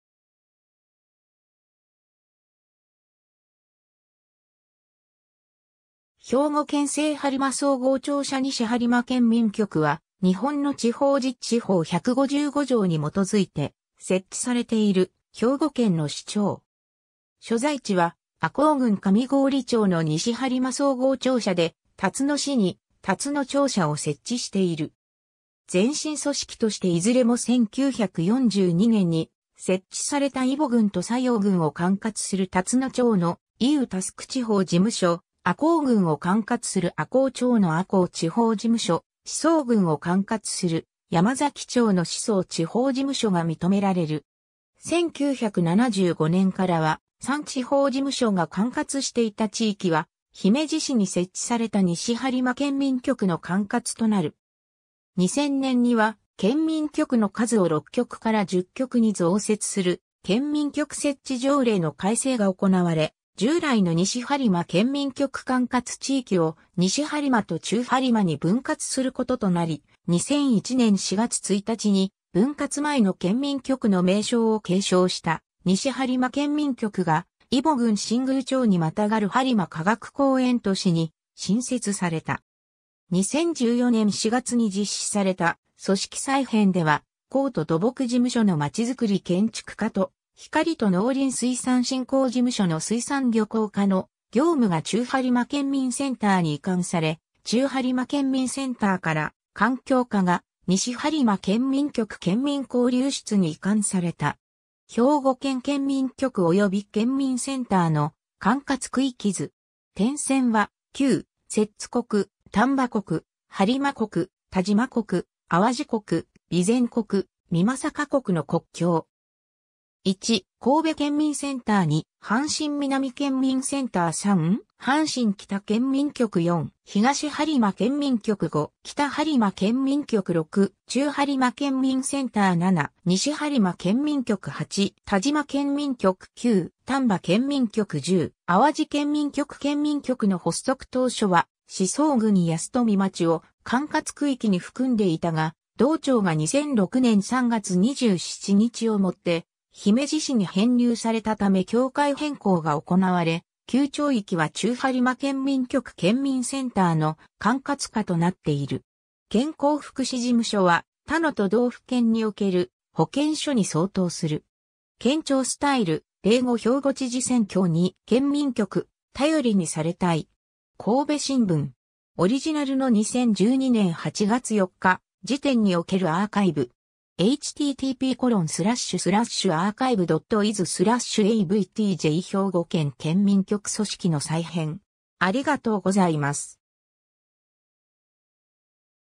西播磨県民局
兵庫県の県民局・県民センター
兵庫県西播磨総合庁舎 西播磨県民局（にしはりまけんみんきょく）は日本の地方自治法155条に基づいて設置されている兵庫県の支庁。
 所在地は赤穂郡上郡町（播磨科学公園都市）の西播磨総合庁舎で、たつの市に龍野庁舎を設置している。
 前身組織としていずれも1942年（昭和17年）に設置された揖保郡と佐用郡を管轄する龍野町の揖佐（いっさ）地方事務所、赤穂郡を管轄する赤穂町の赤穂地方事務所、宍粟郡を管轄する山崎町の宍粟地方事務所が認められる。
 1975年（昭和50年）からは、3地方事務所が管轄していた地域は姫路市に設置された西播磨県民局（第1次）の管轄となる。
 2000年（平成12年）には県民局の数を6局から10局に増設する県民局設置条例の改正が行われ、従来の西播磨県民局管轄地域を西播磨と中播磨に分割することとなり、2001年（平成13年）4月1日に分割前の県民局の名称を継承した西播磨県民局（第2次）が揖保郡新宮町（現たつの市)、赤穂郡上郡町、佐用郡三日月町（現佐用町）にまたがる播磨科学公園都市に新設された。
 2014年（平成26年）4月に実施された組織再編では、光都土木事務所のまちづくり建築課と光都農林水産振興事務所の水産漁港課の業務が中播磨県民センター（旧県民局）に移管され、中播磨県民センターから環境課が西播磨県民局県民交流室に移管された。
 兵庫県県民局及び県民センターの管轄区域図。
点線は旧・摂津国、丹波国、播磨国、但馬国、淡路国、備前国、美作国の国境（幕末期）。
 1.□神戸県民センター 2.■阪神南県民センター 3.■阪神北県民局 4.■東播磨県民局 5.■北播磨県民局 6.■中播磨県民センター 7.■西播磨県民局 8.■但馬県民局 9.■丹波県民局 10.■淡路県民局 県民局の発足当初は宍粟郡安富町を管轄区域に含んでいたが、同町が2006年（平成18年）3月27日を以て姫路市に編入されたため境界変更が行われ、旧町域（姫路市安富町）は中播磨県民局→県民センターの管轄下となっている。
 健康福祉事務所は他の都道府県における保健所に相当する。
 ^ “県庁すたいる―'05兵庫知事選挙 2.県民局「頼りにされたい…」”. 神戸新聞. (2005年6月27日). オリジナルの2012年8月4日時点におけるアーカイブ。
^ 兵庫県県民局組織の再編